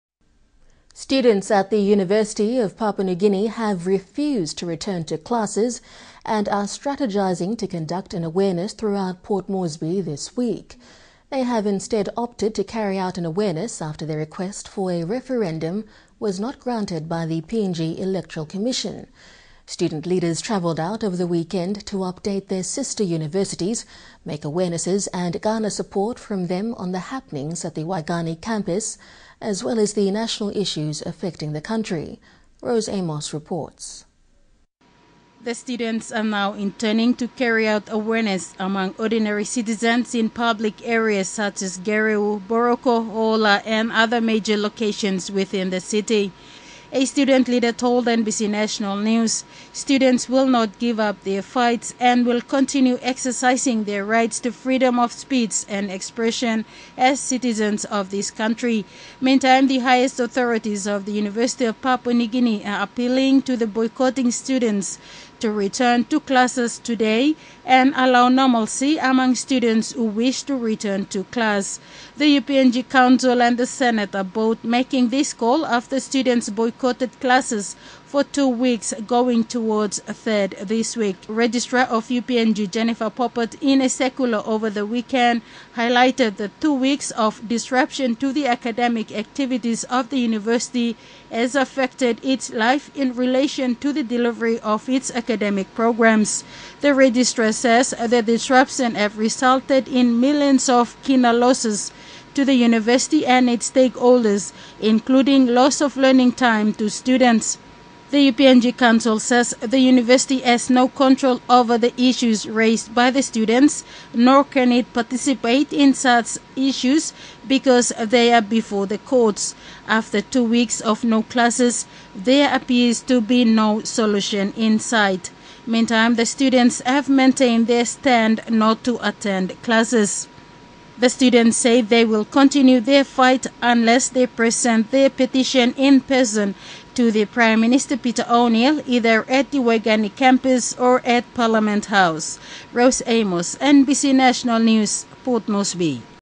UPNG Appeal_NBC News
Students at the University of Papua New Guinea have refused to return to classes and are strategizing to conduct awareness throughout Port Moresby this week.